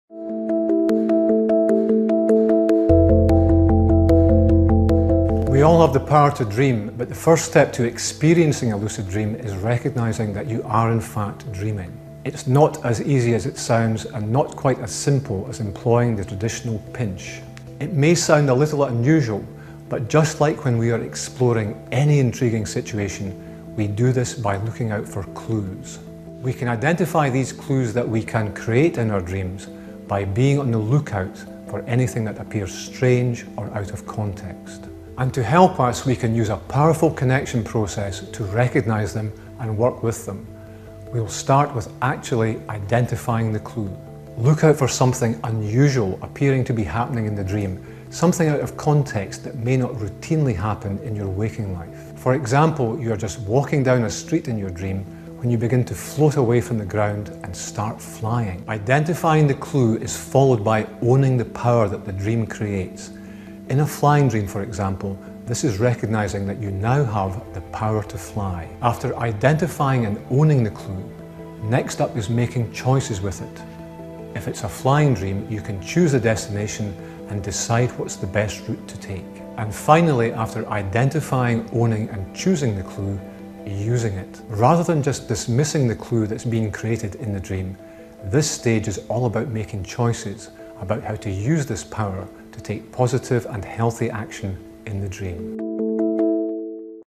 BBC iWonder Lucid Dreams
Do I have the power to control my dreams?

How do I recognize a lucid dream?
We all have the power to dream. But the first step to experiencing a lucid dream is recognising that you are in fact dreaming. It’s not as easy as it sounds and not quite as simple as employing the traditional pinch.

It may sound a little unusual, but just like when we are exploring any intriguing situation, we do this by looking out for clues.
We can identify these clues that we can create in our dreams by being on the look-out for anything that appears strange or out of context. And to help us, we can use a powerful connection process to
recognise them and work with them.

We’ll start with actually identifying the clue.
Look out for something unusual appearing to be happening in the dream, something out of context that may not routinely happen in your waking life.

For example, you are just walking down a street in your dream when you begin to float away from the ground and start flying.
Identifying the clue is followed by owning the power that the dream creates. In a flying dream, for example, this is recognising that you now have the power to fly.

After identifying and owning the clue, next up is making choices with it. If it’s a flying dream you can chose the destination and decide what’s the best route to take.

And finally – after identifying, owning and choosing the clue – using it.

Rather than just dismissing the clue that’s been created in the dream, this stage is all about making choices about how to use this power to take positive and healthy action in the dream.